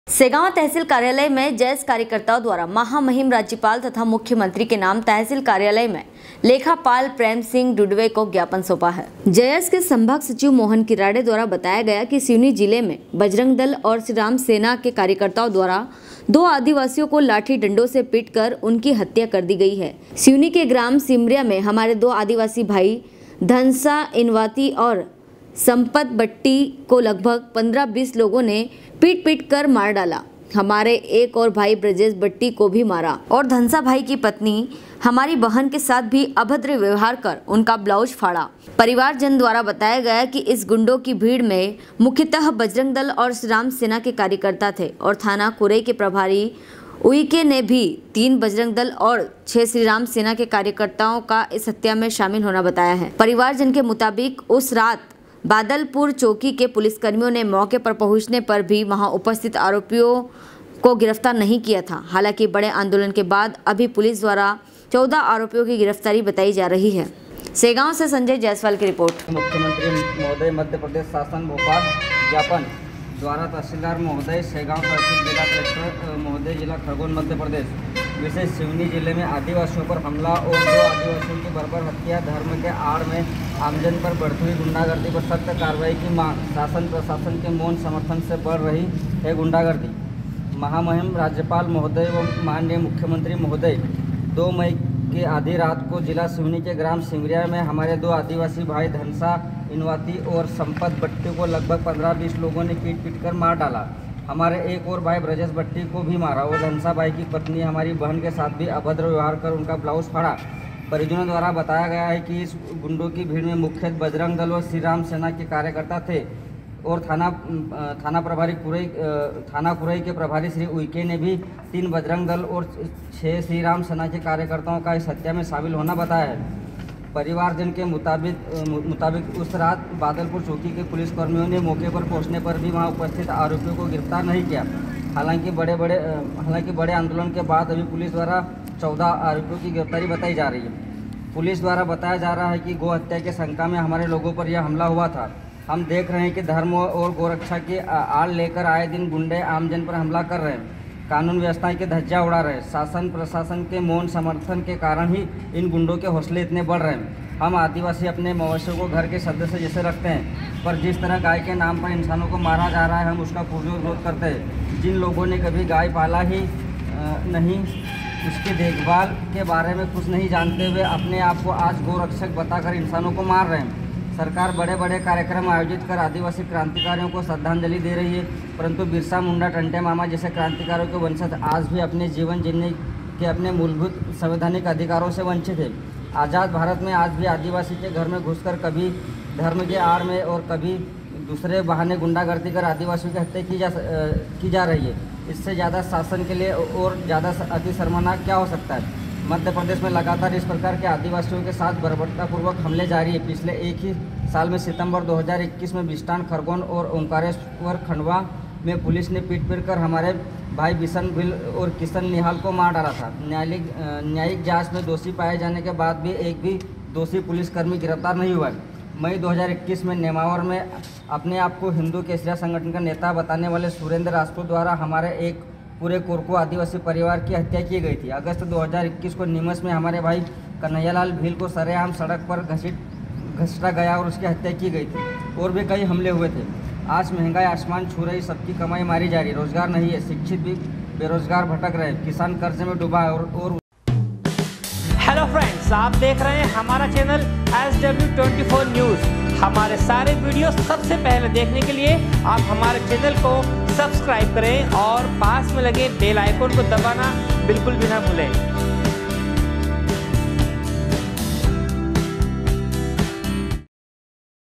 SW 24 News Madhyapradesh
सेगांव तहसील कार्यालय में जयस कार्यकर्ताओं द्वारा महामहीम राज्यपाल तथा मुख्यमंत्री के नाम तहसील कार्यालय में लेखापाल प्रेमसिंह डुडवे को ज्ञापन सौंपा है। सेगांव से संजय जायसवाल की रिर्पोट